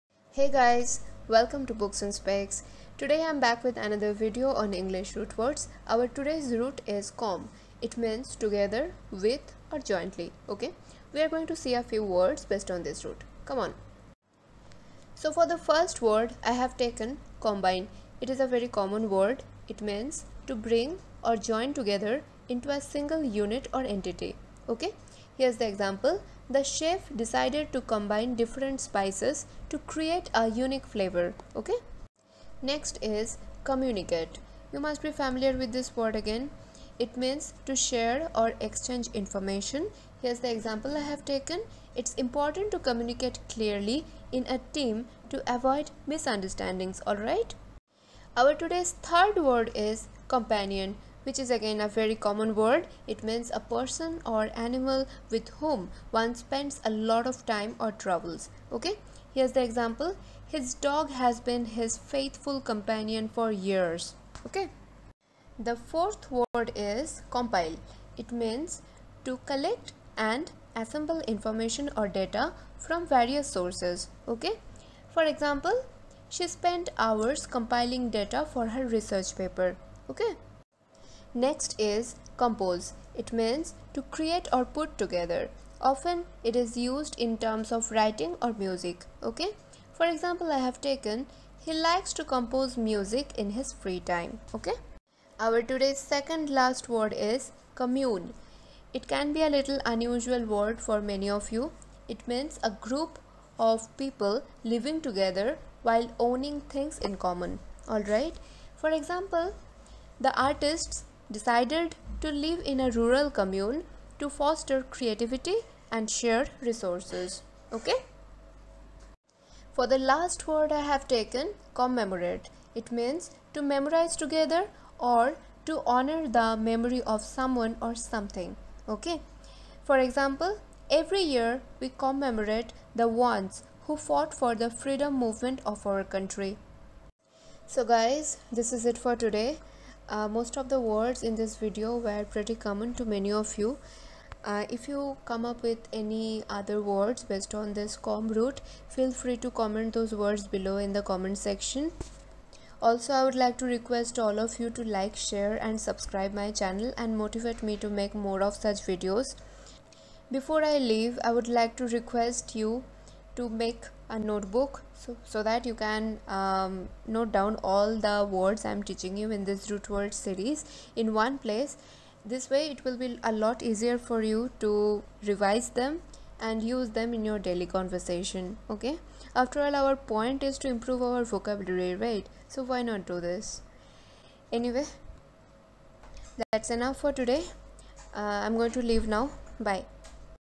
Root words in english vocabulary| Day-51|english vocabulary advanced|new words to improve vocabulary
Hi guys!
Welcome to BooksN'Specs.
This video is about Com root which means together, with, jointly. I have added 10 new words in english with meaning for the audience in this video. Please, revise the words from my earlier video before you watch this one. After all, you will have to keep in mind all these words if you want to improve your vocabulary. So, keep studying. Boost your english speaking practice with these root words in english whether there is an exam ahead or not.
Enjoy the video and don't forget to keep notes.

BooksN'Specs